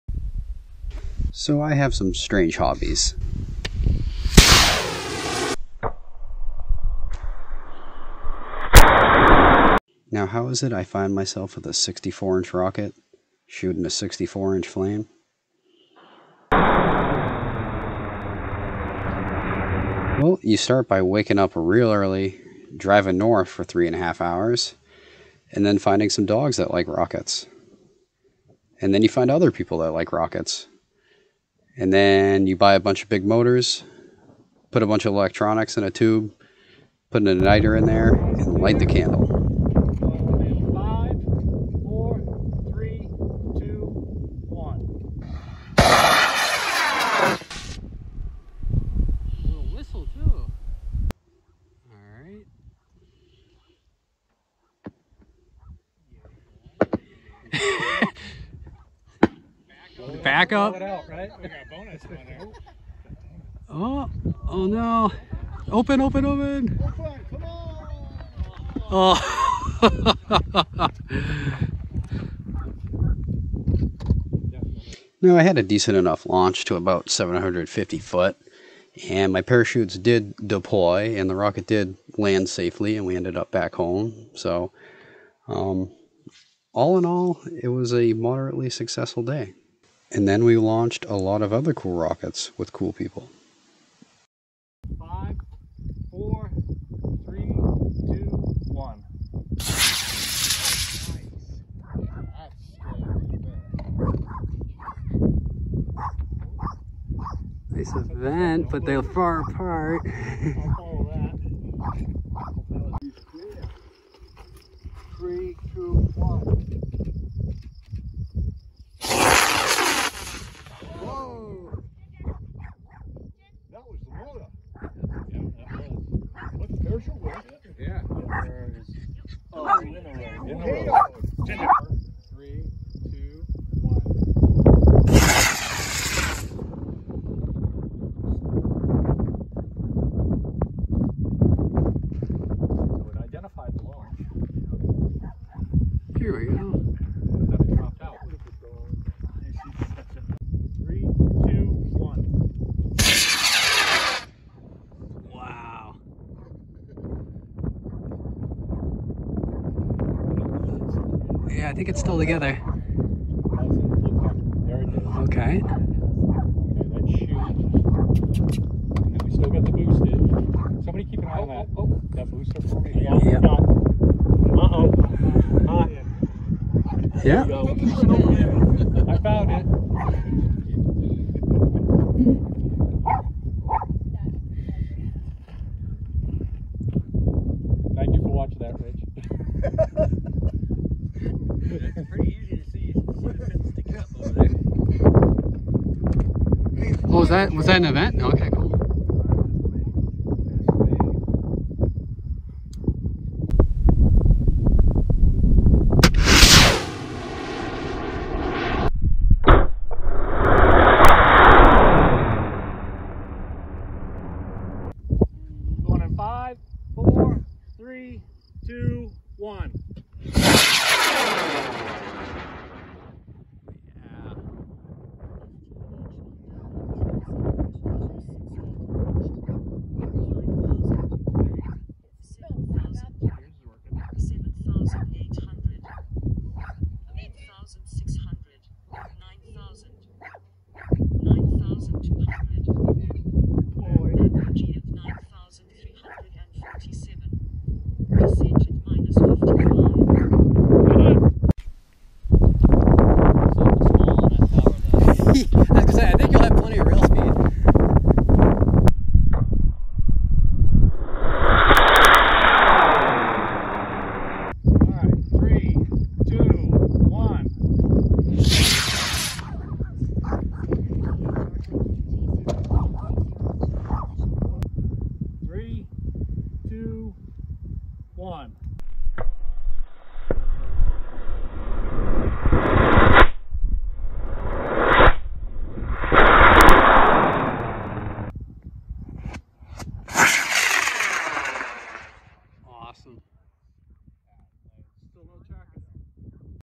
Tripoli Cherryfield May 29, 2022
I traveled up to Cherryfield, Maine to launch my 7.5" LOC Doorknob on a CTI J381 Skidmark for my L2 Certification.

Bonus footage of some other cool rockets.